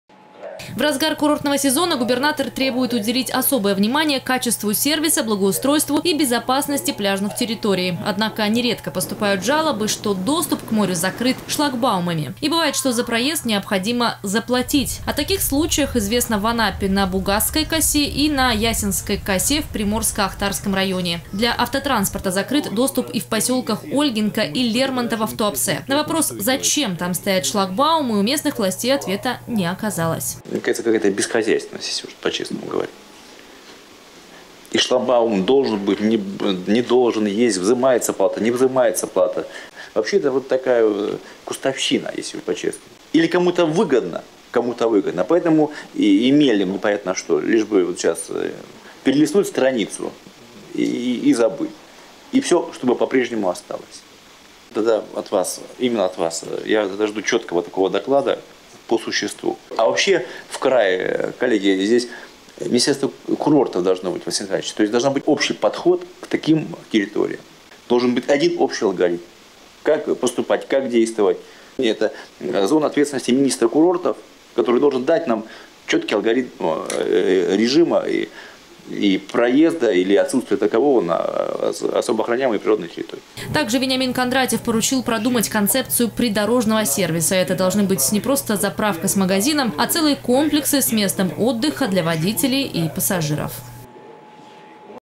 Состояние пляжей и набережных обсудили в краевой администрации. Новости Эфкате
Этот вопрос Вениамин Кондратьев поднял на традиционном «Часе контроля». На совещании глава региона проверил, как исполняют его поручения.

В разгар курортного сезона губернатор требует уделить особое внимание качеству сервиса, благоустройству и безопасности пляжных территорий. Однако нередко поступают жалобы, что доступ к морю закрыт шлагбаумами. И бывает, что за проезд необходимо заплатить. О таких случаях известно в Анапе на Бугазской косе и на Ясенской косе в Приморско-Ахтарском районе. Для автотранспорта закрыт доступ и в поселках Ольгинка и Лермонтово в Туапсе. На вопрос, зачем там стоят шлагбаумы у местных властей ответа не оказалось.